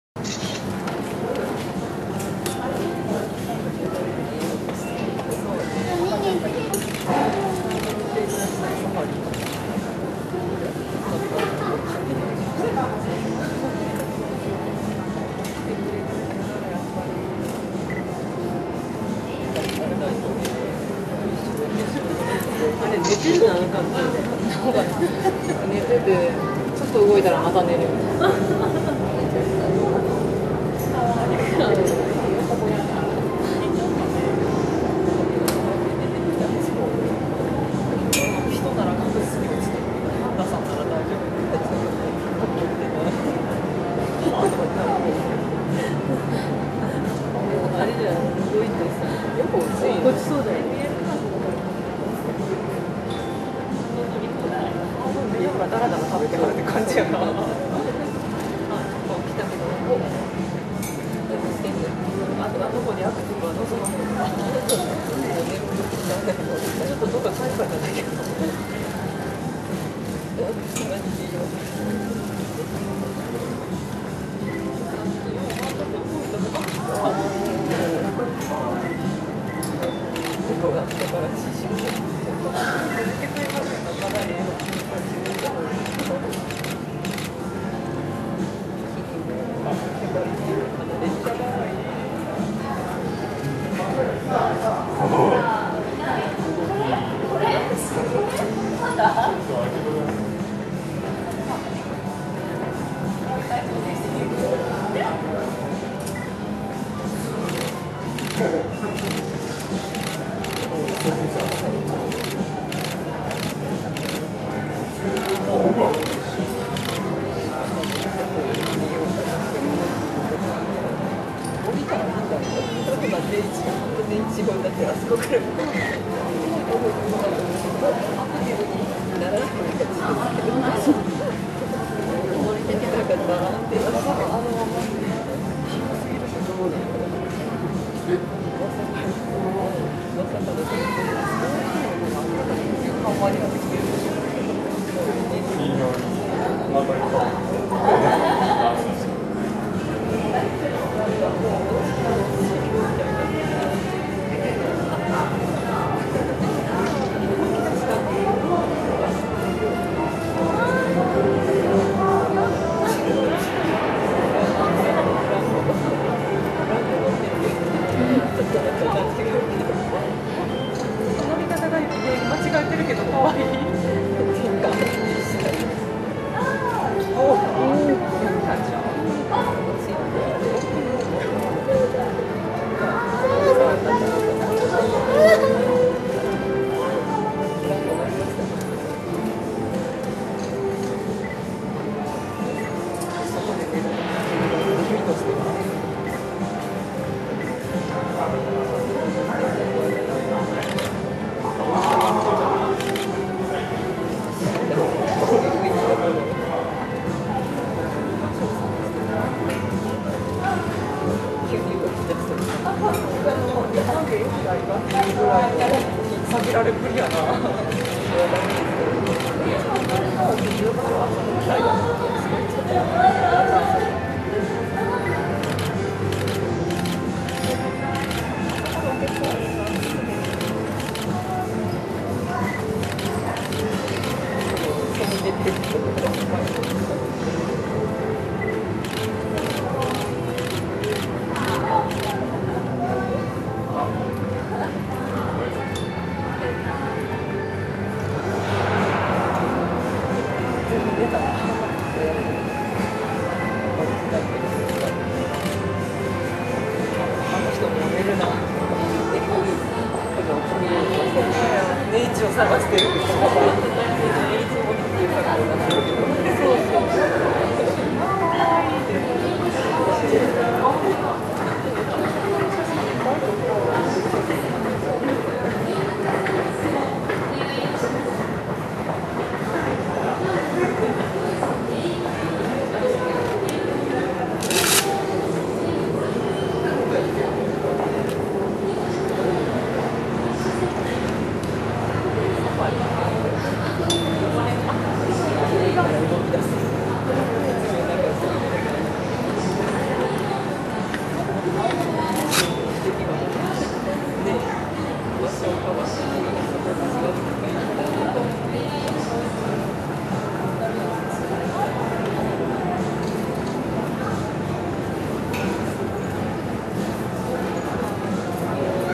Giant Panda Twin Cubs Kaihin and Youhin of 2011 October at Wakayama, Japan #1-13
Youhin, Kaihin Rauhin, twin babies born in Japan, 陽浜＆海浜 白浜 アドベンチャーワールド Youhin and Kaihin were 15-month old then.  They have just started to live without their mom.